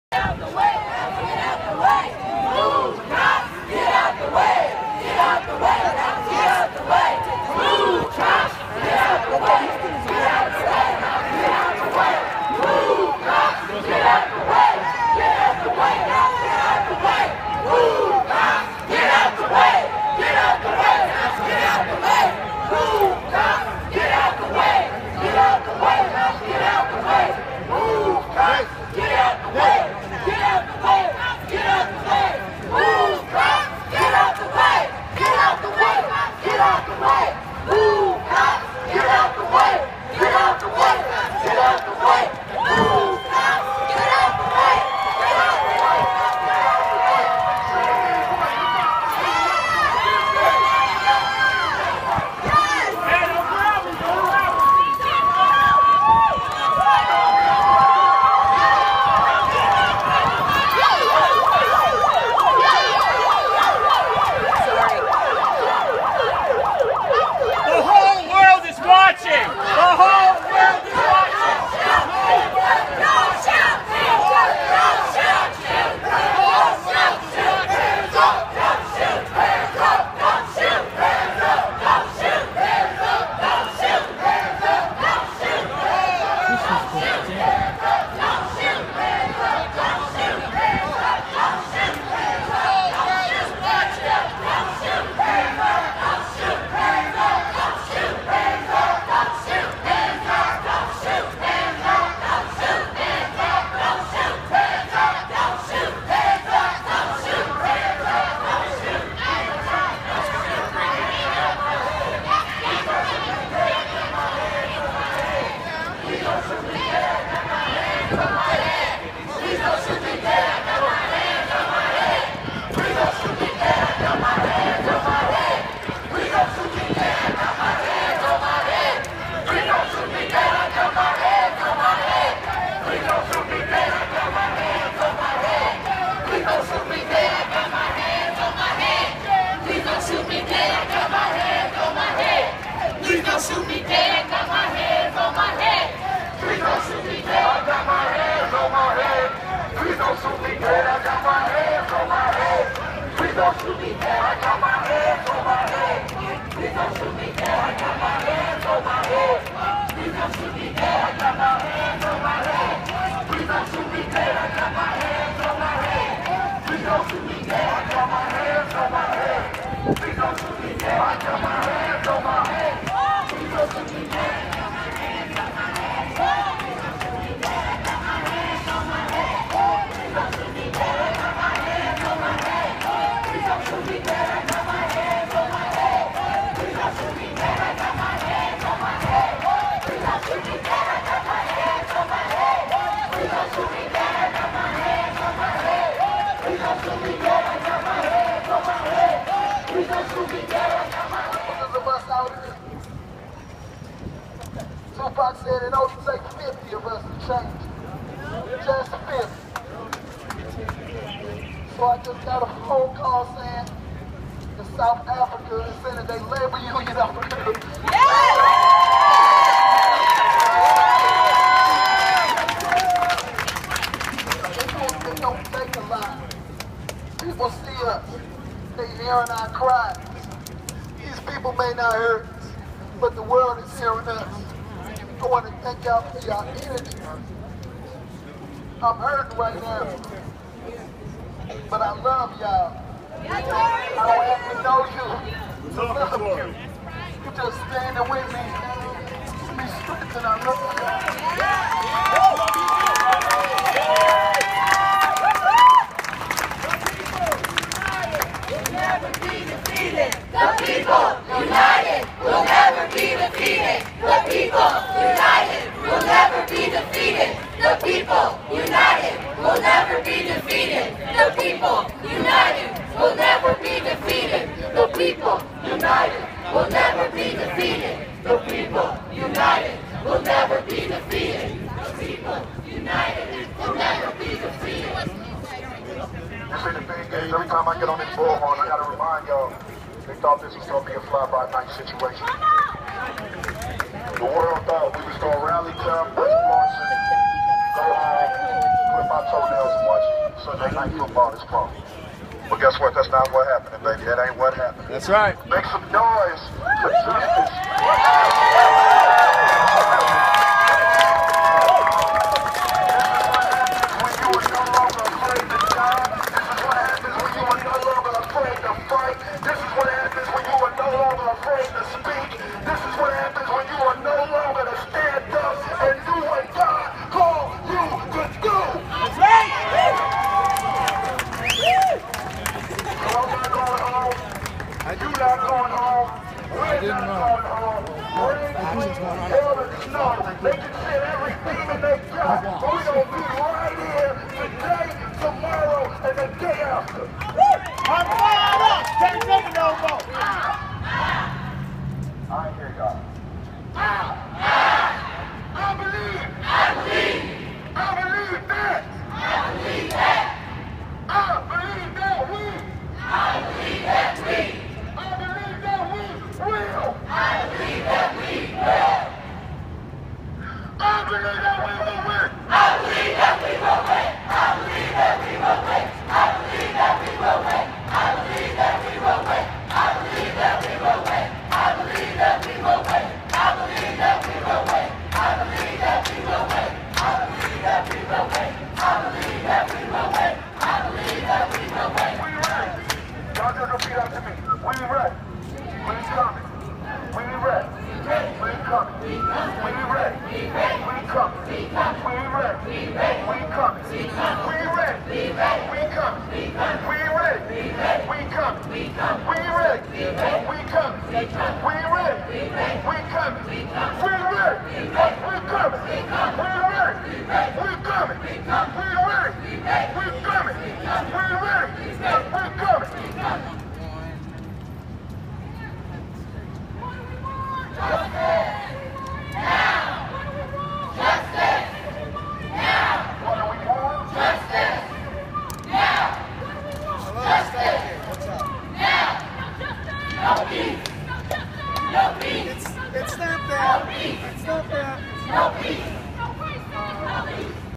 "Move Cops! Get out the way!"
Missouri Organization for Reform and Empowerment MORE lead a protest against police abuse and the killing of Michael Brown and Kajieme Powell at noon on Tuesday, August 26th, 2014. MORE was joined by Jobs with Justice, the Black Panther, Citizens against Police Crimes and Repression and many other progressive activist, community leaders as well as participants from around the world. A reporter from Telesur, a South American news station was there. Kajieme Powell was killed by St. Louis police officer after he waved a knife a them. The protester believed both shootings were an example of police abuse. The marched from St. Louis City hall to the U.S Justice department's Thomas Eagleton building. The demanded Justice for Mike Brown and for Kajieme Powell by marching to City Hall and to the Department of Justice.